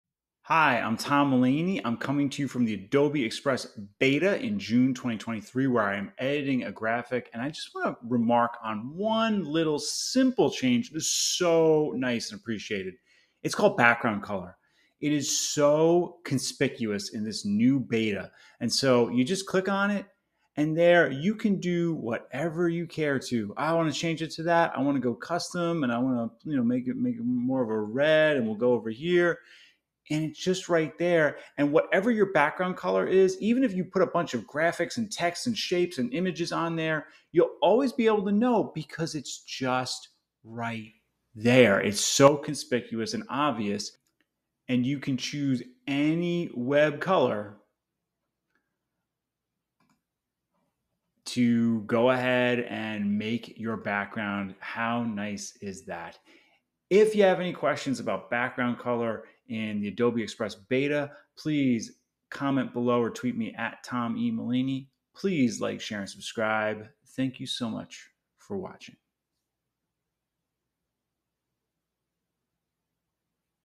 Adobe Express - Background Color
Check out the background color feature in the Adobe Express Beta.